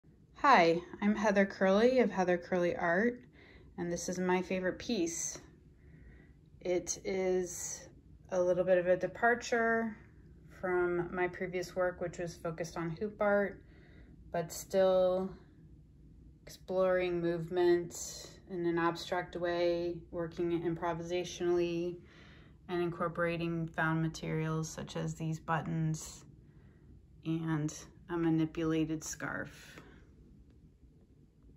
My Favorite Piece: Heather Kerley Art
I try to include a new centerpiece work for every marketplace event. This is my new abstract wall hanging available for sale. This piece represents a new evolution in my work. Appropriately, the stitching was inspired by natural processes, creation, metamorphosis, tendrils reaching out, touching, dying, decaying, everything in flux.